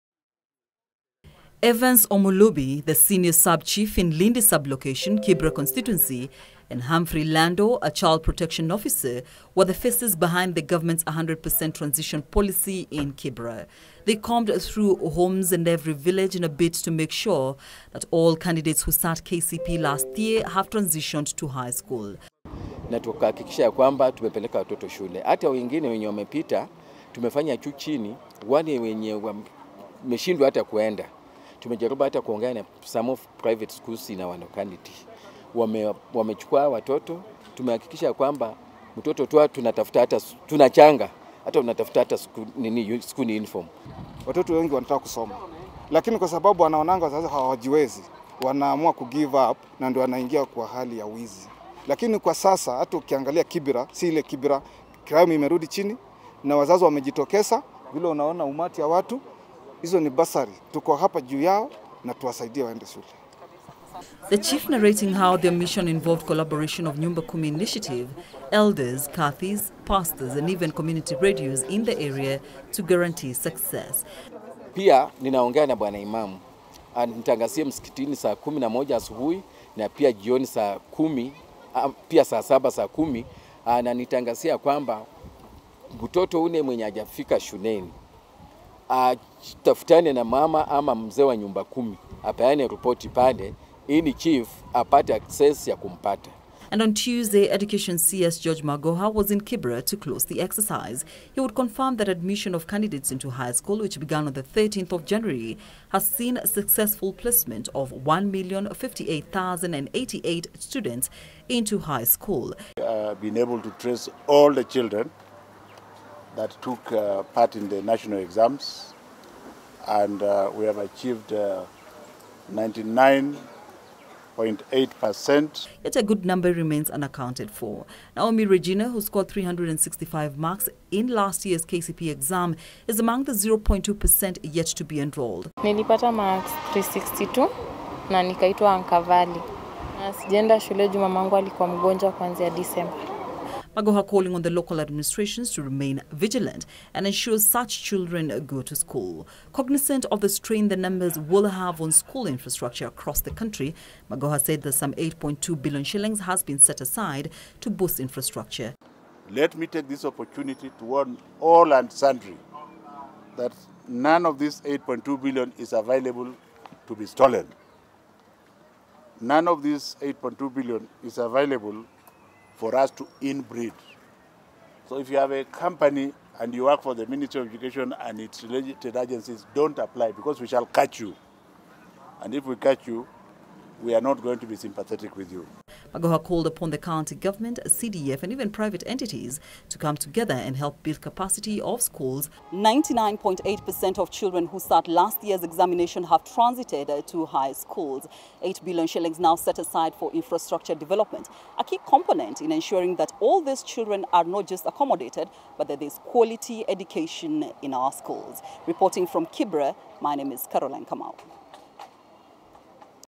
Magoha on 100% transition
Some 4,811 candidates who sat the 2019 Kenya Certificate of Primary Education cannot be accounted for. This is after the government disclosed that the 100 % enrolment to secondary schools achieved 99.8% transition representing 1,058,088 candidates. Speaking has he officially closed the form one mop-up exercise, Education Cabinet Secretary George Magoha noted that a total of 8 billion shillings have also been set aside to boost infrastructure in high schools so as to ensure that all those enrolled are facilitated to learn smoothly. Caroline Kamau reports.